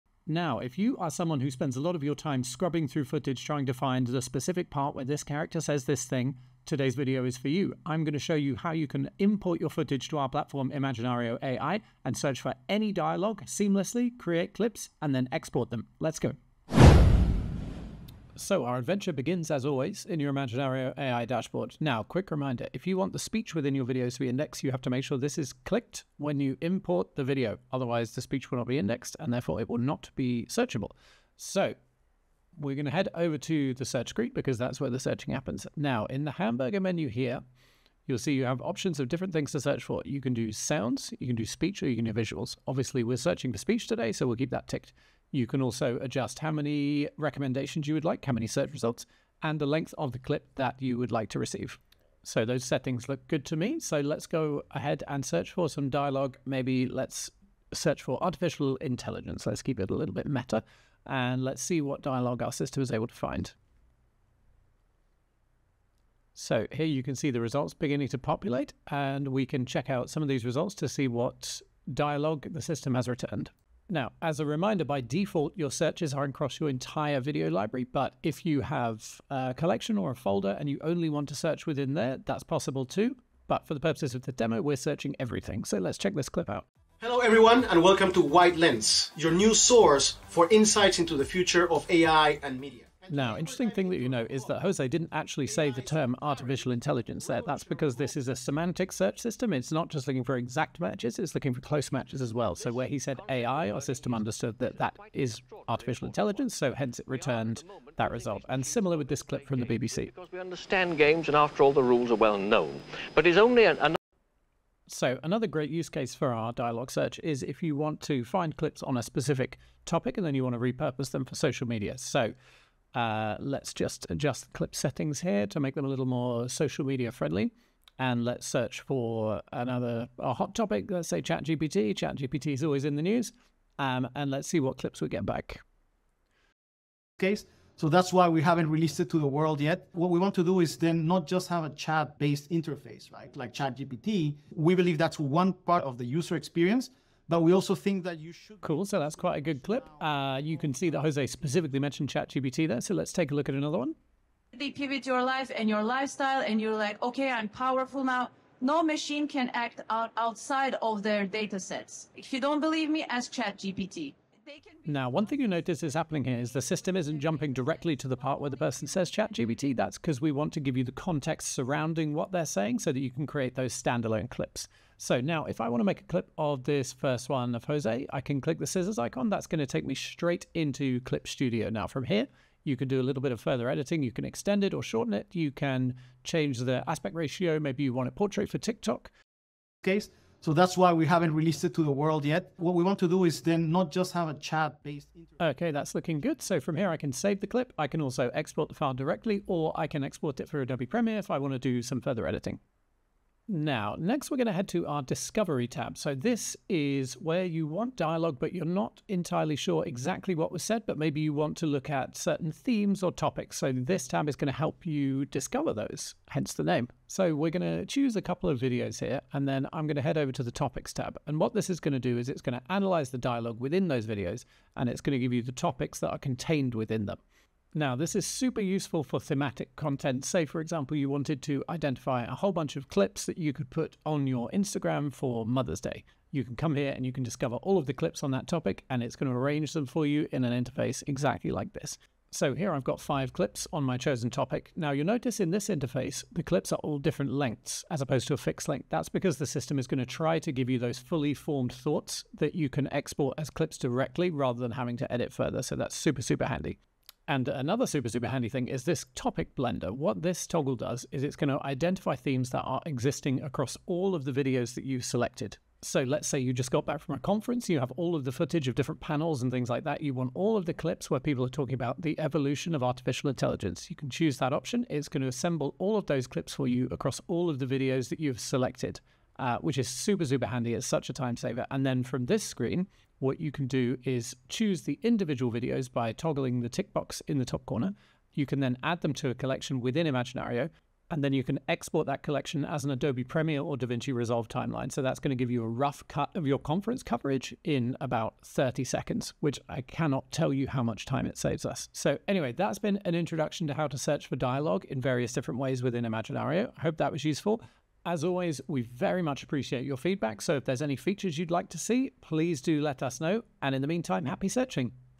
How to search for dialogue, speech and conversation topics within your videos
Learn how to use Imaginario AI to search inside your video content for any spoken material. Search for specific words or lines, themes, or entire conversation topics.

Preview your results, create clips for social media, or export for further editing.

00:00 → Intro
00:17 → Dashboard overview and indexing options
00:33 → Search tab and search options
1:00 → Searching for dialogue
1:56 → Searching by topic
2:54 → Editing in Clip Studio
3:23 → The Discovery tab
3:41 → Creating clips with topics
4:16 → Creating compilations with blended topics